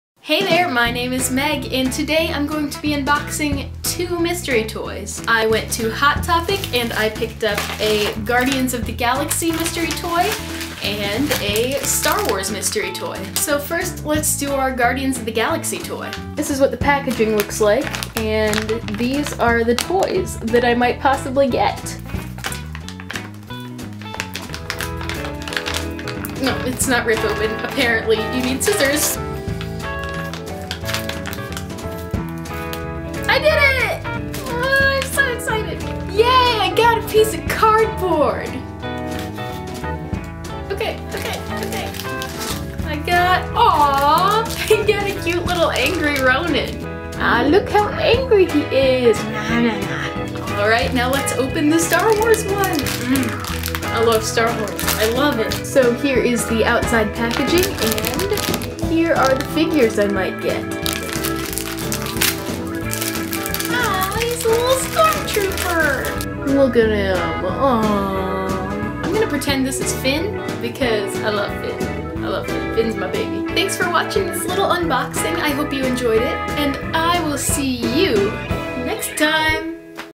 Mystery Toy Unboxing! (Star Wars and Guardians of the Galaxy)
Just a lil unboxing video of the mystery toys I got at Hot Topic!